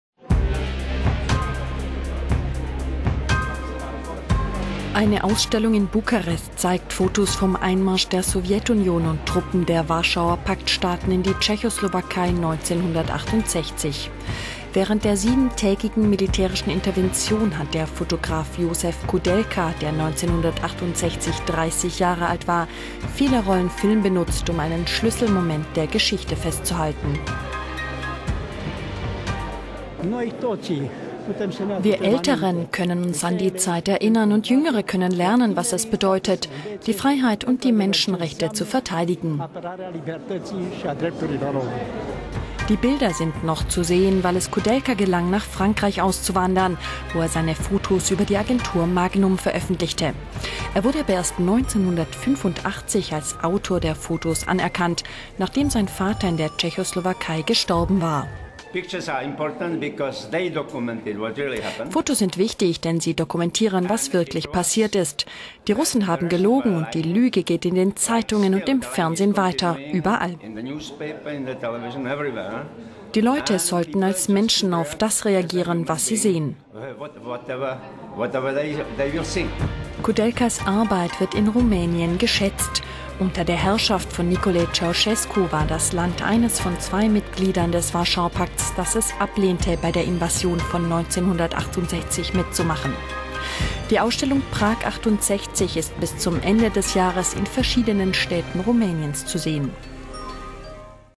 Ausstellung: Josef Koudelka zeigt Fotos vom Einmarsch in die Tschechoslowakei 1968 - le mag
Eine Ausstellung in Bukarest zeigt Fotos vom Einmarsch der Sowjetunion und Truppen der Warschauer...

Eine Ausstellung in Bukarest zeigt Fotos vom Einmarsch der Sowjetunion und Truppen der Warschauer Pakt-Staaten in die Tschechoslowakei 1968.

Während der siebentägigen militärischen Intervention hat der Fotograf Josef Koudelka, der 1968 30 Jahre alt war, viele Rollen Film benutzt, um einen Schlüsselmoment der Geschichte festzuhalten.

Ausstellungsbesucher:
"Wir Älteren können uns an die Zeit erinnern, und Jüngere können lernen, was es bedeutet, die Freiheit und die Menschenrechte zu verteidigen."

Die Bilder sind noch zu sehen, weil es Koudelka gelang, nach Frankreich auszuwandern, wo er seine Fotos über die Agentur Magnum veröffentlichte. Er wurde aber erst 1985 als Autor der Fotos anerkannt, nachdem sein Vater in der Tschechoslowakei gestorben war.

Josef Koudelka:
"Fotos sind wichtig, denn sie dokumentieren, was wirklich passiert ist. Die Russen haben gelogen, und die Lüge geht in den Zeitungen und dem Fernsehen weiter, überall. Die Leute sollten als Menschen auf das reagieren, was sie sehen."

Koudelkas Arbeit wird in Rumänien geschätzt. Unter der Herrschaft von Nicolae Ceausescu war das Land eines von zwei Mitgliedern des Warschauer Pakts, das es ablehnte, bei der Invasion von 1968 mitzumachen.